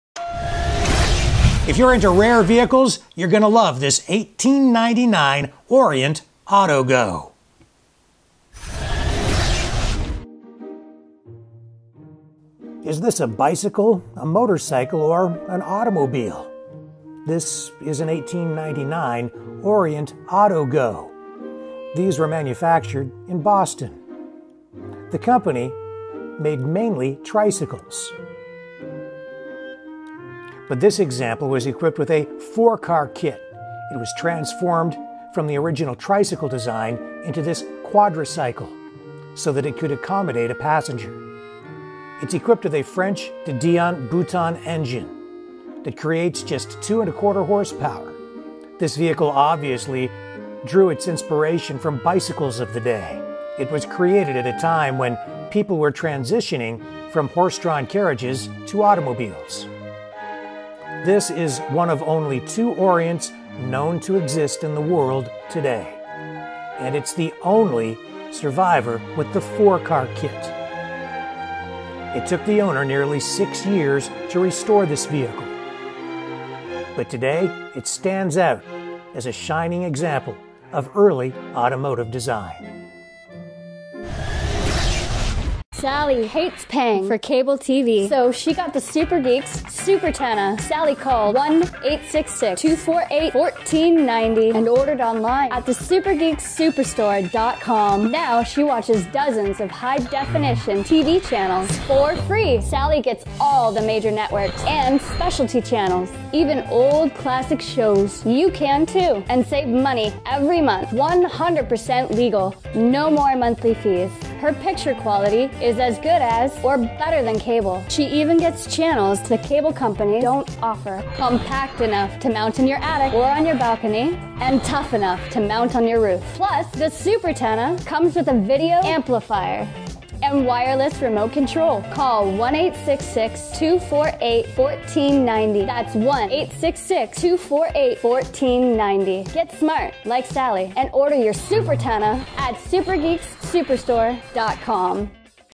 World's Last Surviving 1899 Orient AutoGo Quadricycle - SST CAR SHOW
Last surviving 1899 Orient AutoGo Quadricycle. These were produced in BOSTON and were among the first vehicles to bridge the gap between horse drawn carriages and modern automobiles. This quadricycle could be driven with pedal power, or a small internal combustion engine.

This restored 1899 Orient AutoGo was at the Greenfield Village old Car Festival, and has appeared at some of North America's most prestigious automotive events.

The Orient tricycle was an early motorized tricycle (classified as a motorcycle under some definitions). It was manufactured by Charles H. Metz's Waltham Manufacturing Company in Waltham, Massachusetts and advertised in 1899 as a "motor cycle", the first use of the term in a published catalog.[1]

Orient advertised that the single-person tricycle could be converted to a two-person four wheeled "autogo" in five minutes.[2] A 1900 Orient appeared in The Art of the Motorcycle exhibition at Guggenheim Museum in New York.[3]